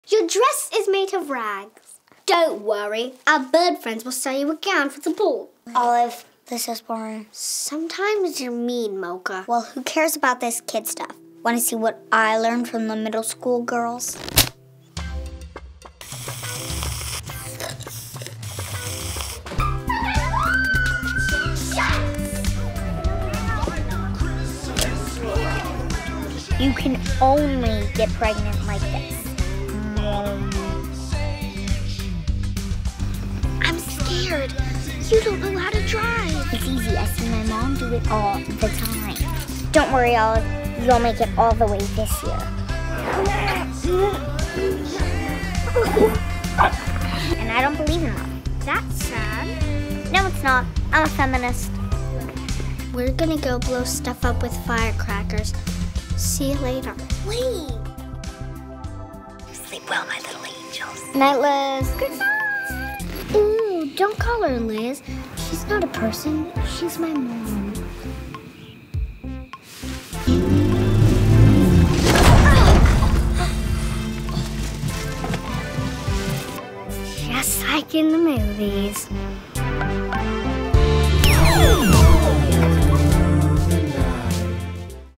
Trailer - 3 new episodes!!
A goody-goody and a bad seed are magnets for mischief in the 80s.